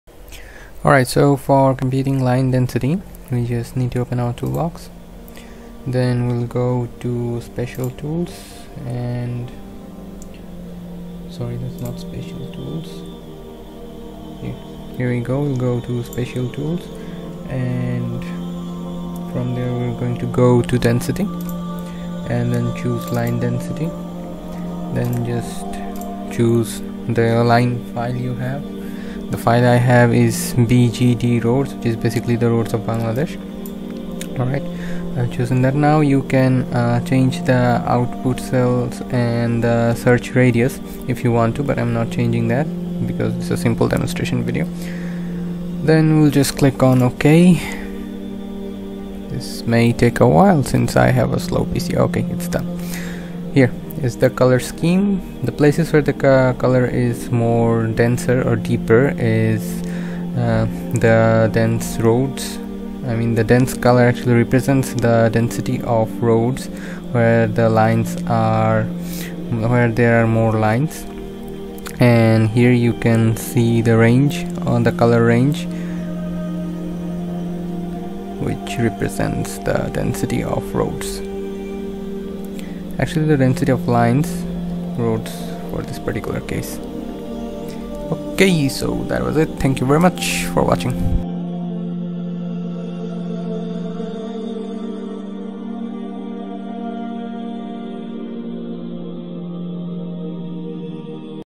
How to calculate Line density in ArcGIS
Learn to Calculate line density in ArcGIS !

Like my FB page to get updates and to participate in quizzes  & Quiz competitions